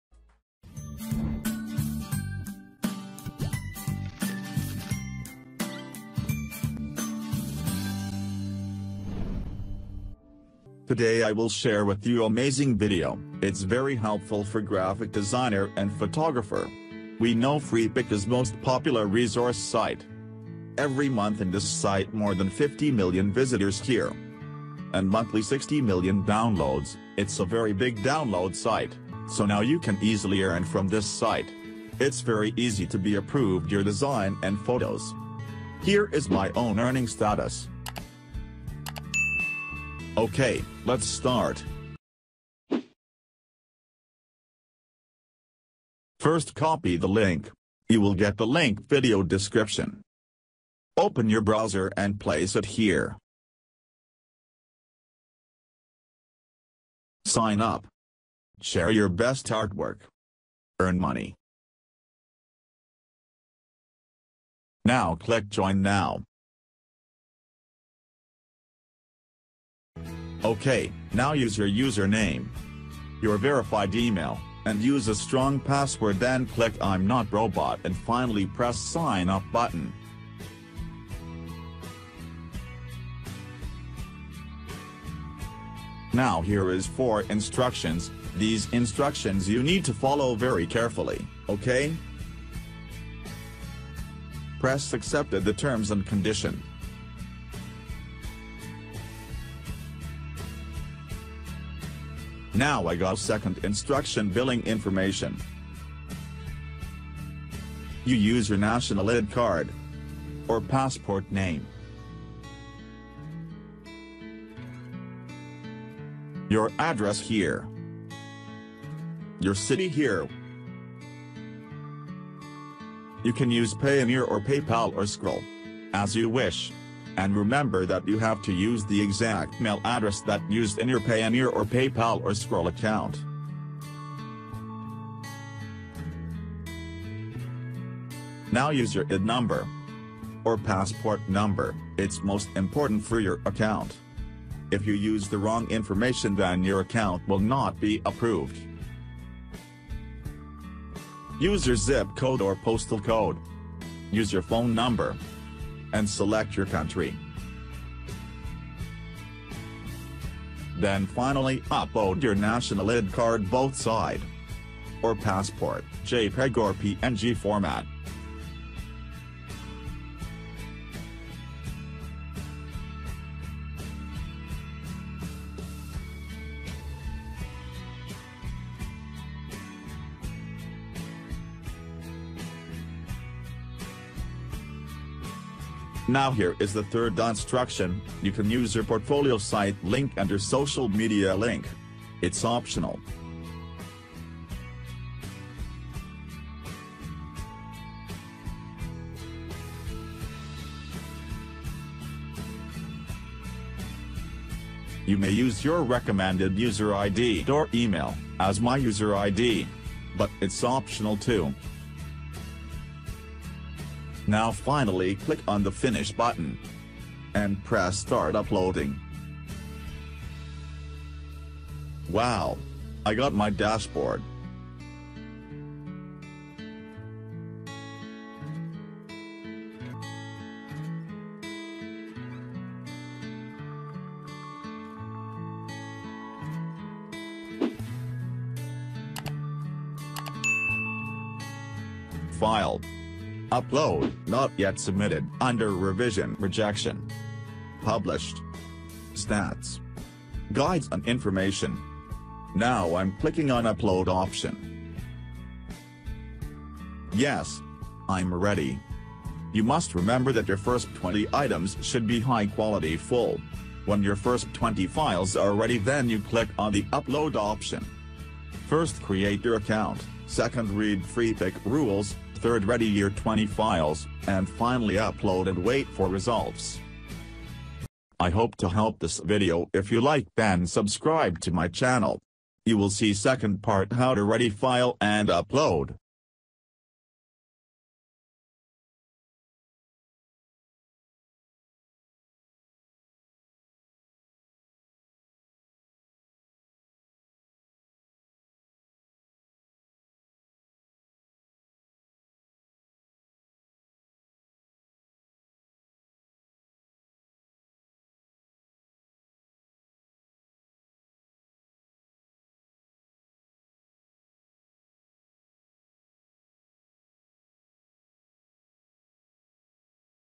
Become a Freepik Contributor | How to Create Freepik Contributor Account | Earn money Freepik
Are you from Bangladesh? Then see my Bangla video
খুব সহজেই আপনি Freepik.com থেকে আয় করতে পারবেন এখন | আপনার কম্পিউটারে ফেলে রাখা বা GraphicRiver এ রিজেক্ট হওয়া ডিজাইনগুলো আপলোড করে ইনকাম করতে পারবেন। খুব সহজেই এখানে Approved করানো যাচ্ছে।